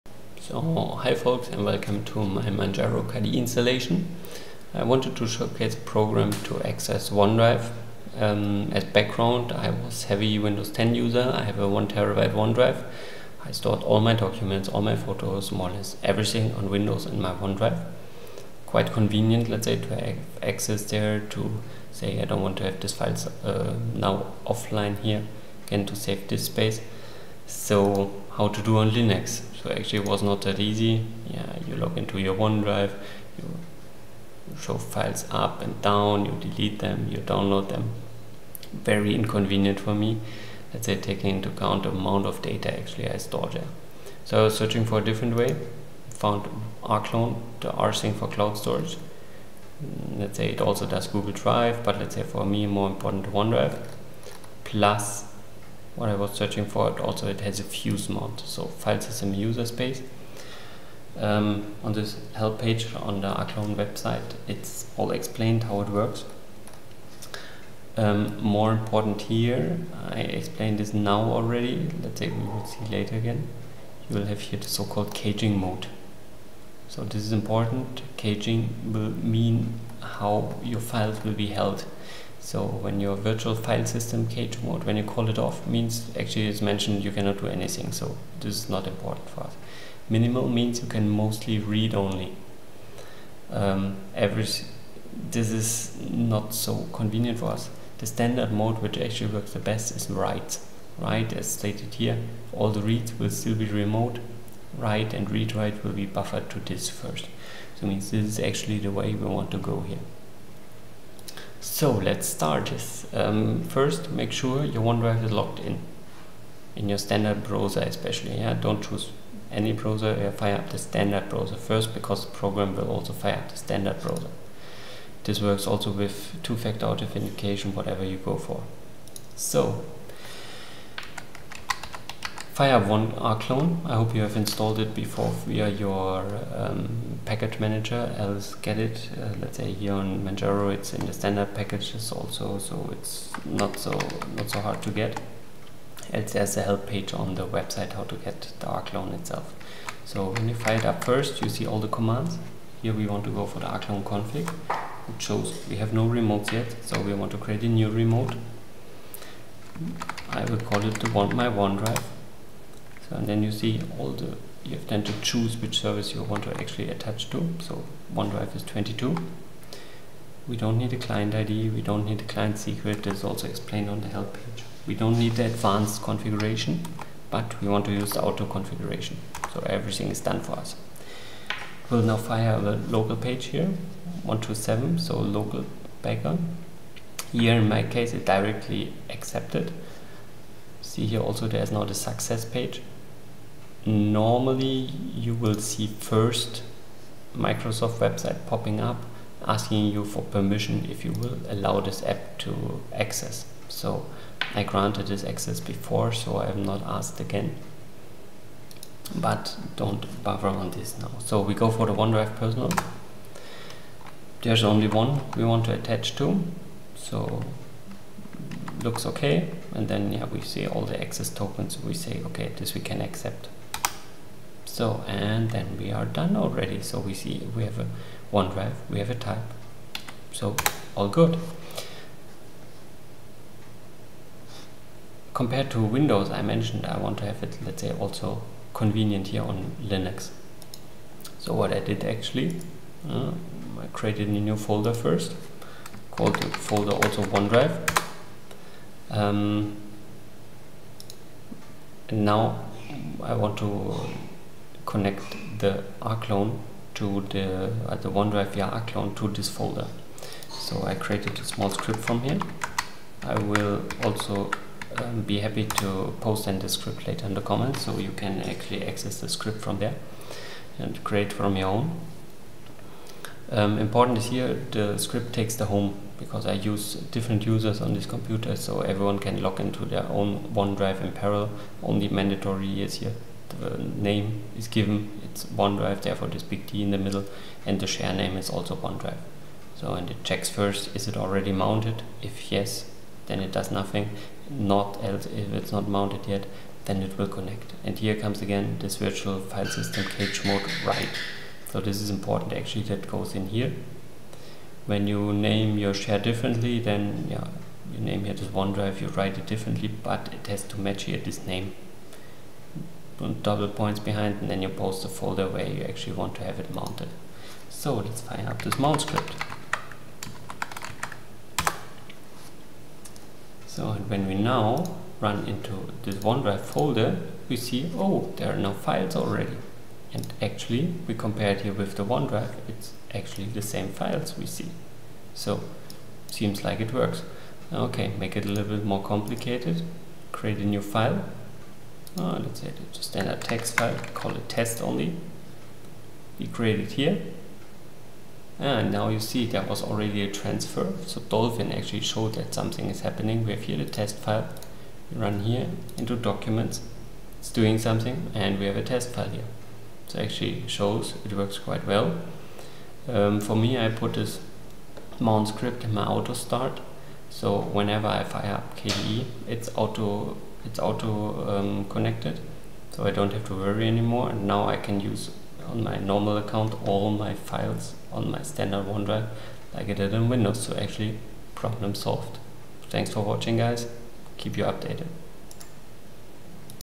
Mount Onedrive in Linux
In this video I explain how to setup rclone to mount Onedrive in Linux to have a similar user experience as with Windows 10.
I show the setup of the account and give some tricks how to get it auto-connected during login.